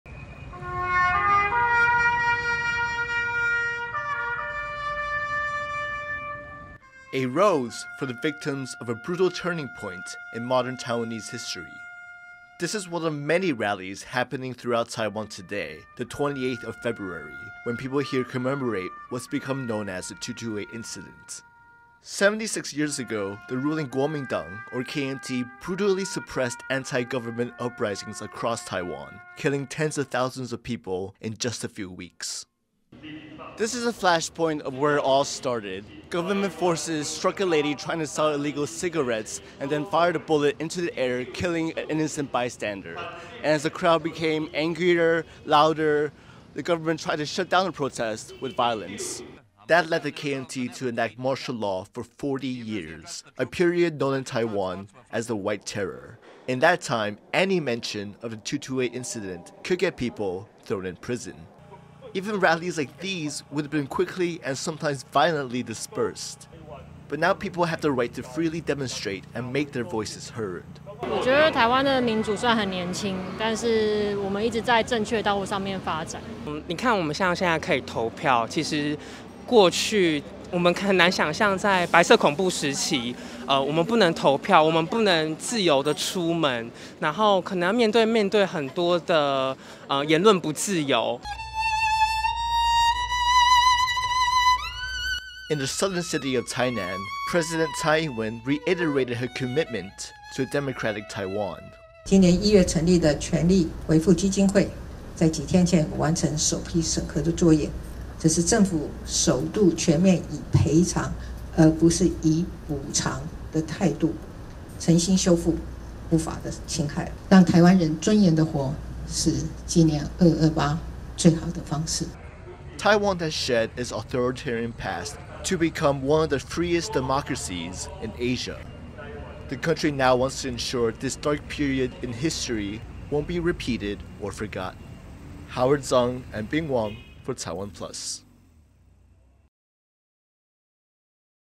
Rallies Across Taiwan Remember 228 Incident | TaiwanPlus News
📹Reporters: Howard Chang/Bing Wang

February 28 is Peace Memorial Day in Taiwan. This year marks the 76th anniversary of the 228 Incident, a massacre that paved the way for 40 years of martial law. People are holding rallies around Taiwan on Tuesday to commemorate the struggles they’ve overcome.